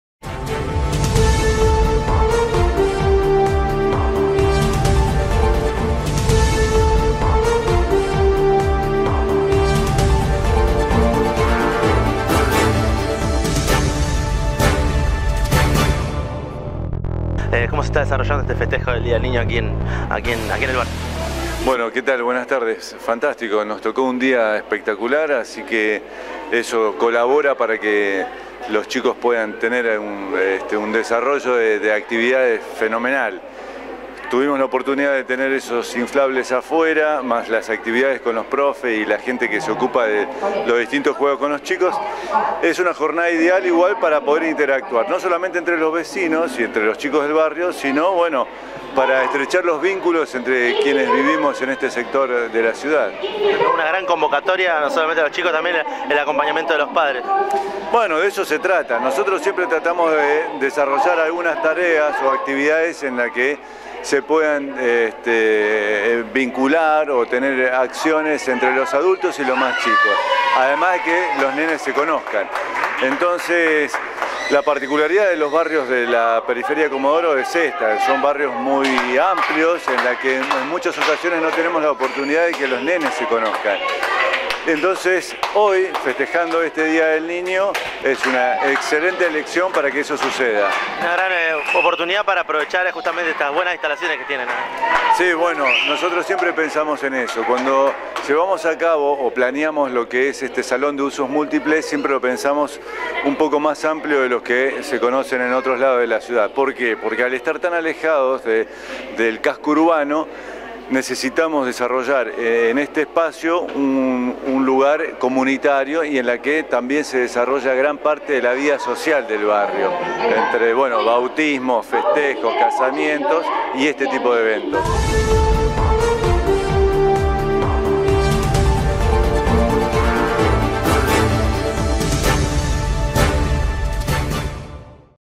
Festejo Día del Niño en Bella Vista Sur
(HD).- En el marco de los festejos del Día del Niño, estuvimos acompañando la jornada en Bella Vista Sur, donde también la Unión Vecinal estuvo a pleno en los festejos junto al Programa Recreación del Ente Comodoro Deportes.
 Los chicos respondieron a la convocatoria en una jornada muy especial el pasado domingo 27 de agosto 2017.
Cámara & Edición: Darío Arias - Periodista: Hernán Romero.